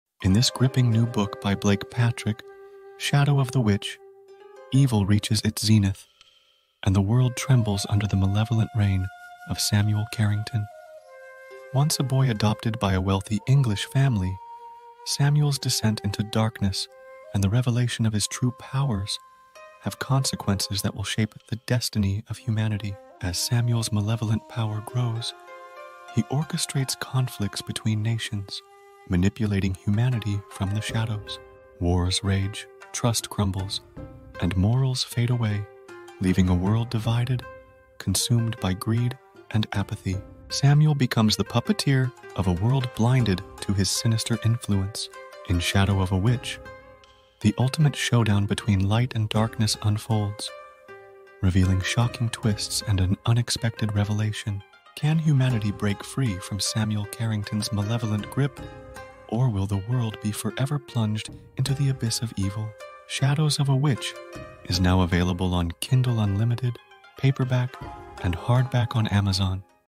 Shadow of a Witch by Blake Patrick
In this gripping new book by Blake Patrick, “Shadow of the Witch”, evil reaches its zenith, and the world trembles under the malevolent reign of Samuel Carrington. Once a boy adopted by a wealthy English family, Samuel's descent into darkness and the revelation of his true powers have consequences that will shape the destiny of humanity.

As Samuel's malevolent power grows, he orchestrates conflicts between nations, manipulating humanity from the shadows. Wars rage, trust crumbles, and morals fade away, leaving a world divided, consumed by greed and apathy. Samuel becomes the puppeteer of a world blinded to his sinister influence.

But within this bleak landscape, a few brave souls refuse to surrender to the darkness. Ds Karl Cushnahan, Isabella, and their allies dare to challenge the malevolence that threatens to engulf the world. The battle between good and evil intensifies, and the fate of Ravenwood Manor hangs in the balance.

In "Shadow of a Witch” the ultimate showdown between light and darkness unfolds, revealing shocking twists and an unexpected revelation. Can humanity break free from Samuel Carrington's malevolent grip, or will the world be forever plunged into the abyss of evil?

Prepare for a heart-pounding, mind-bending journey that will keep you on the edge of your seat. As the pages turn, you'll be captivated by a story of courage, resilience, and the enduring power of hope in the face of overwhelming malevolence.

US Kindle
From these links you can also purchase the paperback and Hardback copies.